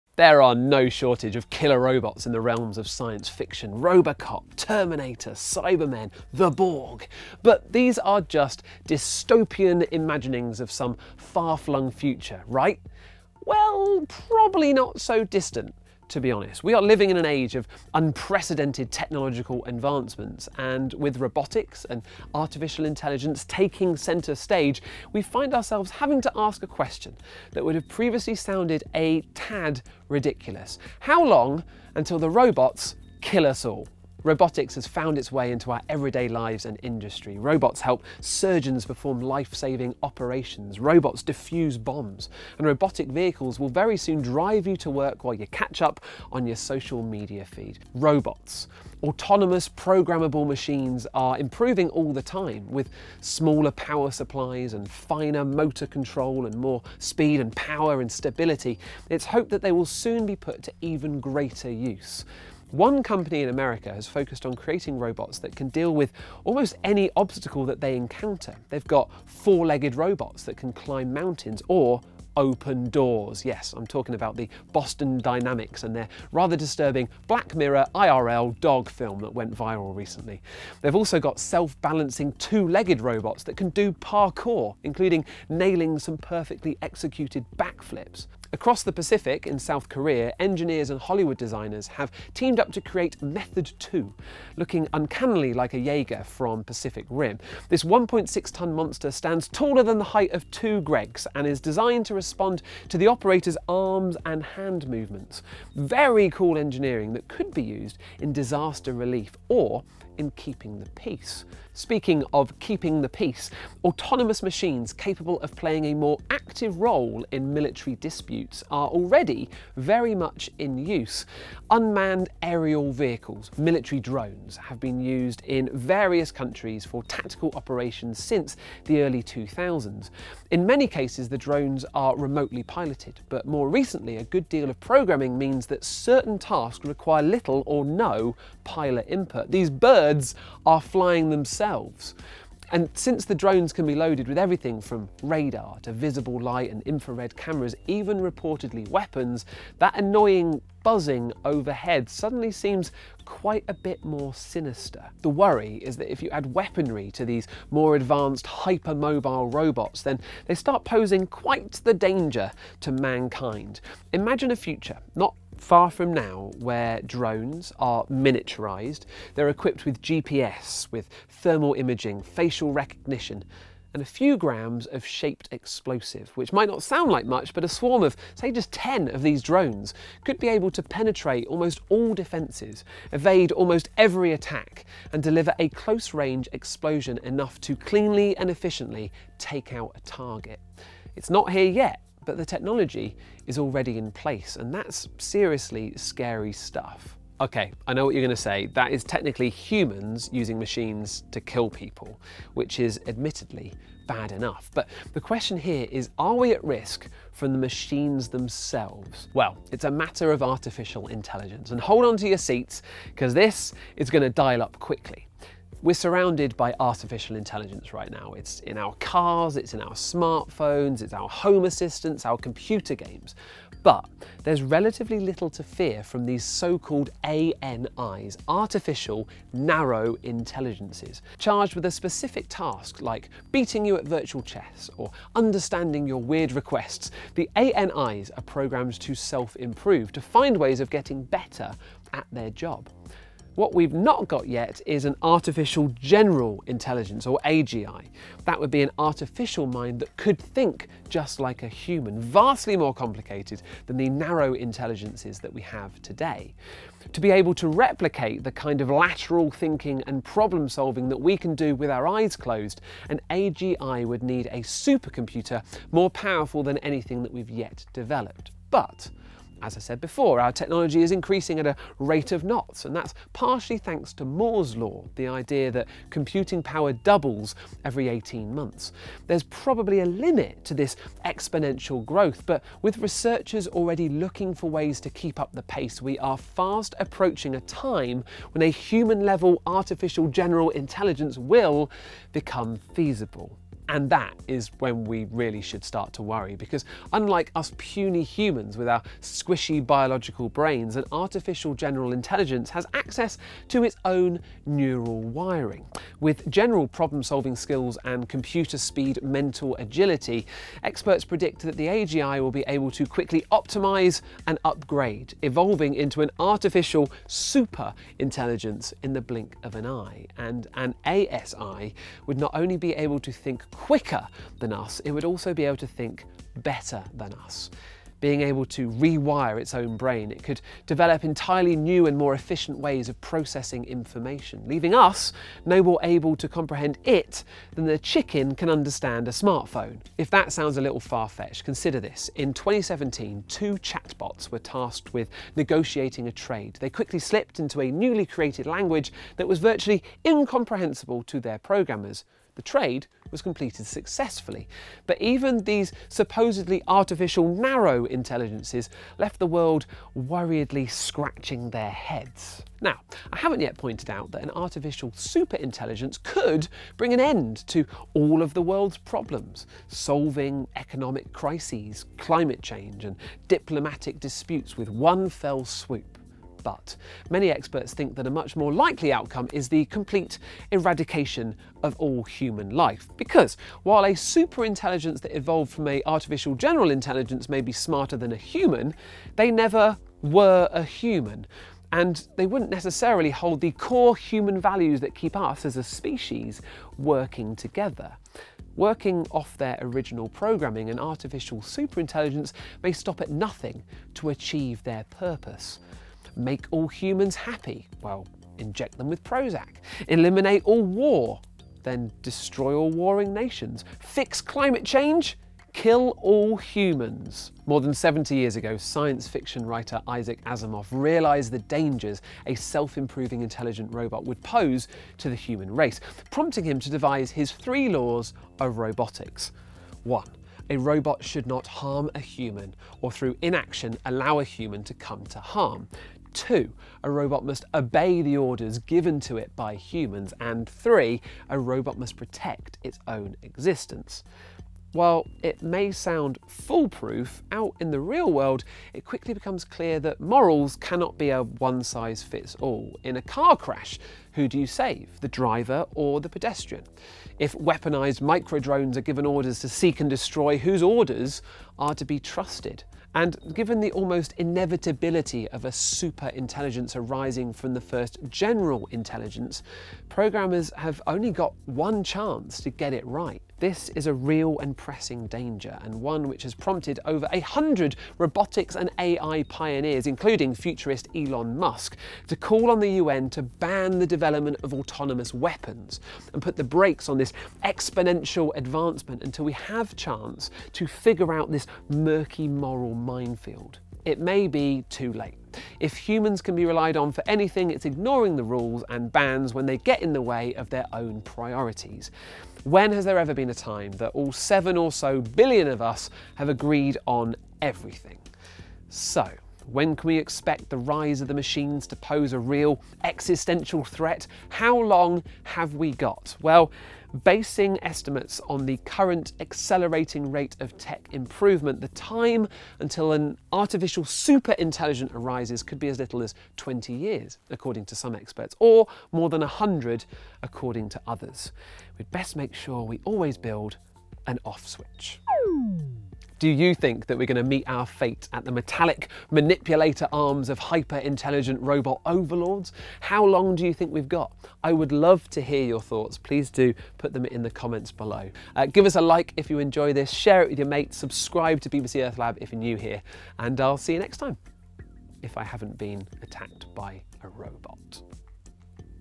When will Robots Kill Us All? | Earth Science
From HAL and Skynet to the Roko's Basilisk thought experiment, artificial intelligence has been depicted as terrifying in popular culture.  Greg Foot looks at whether new technology is moving fast enough for us to be scared.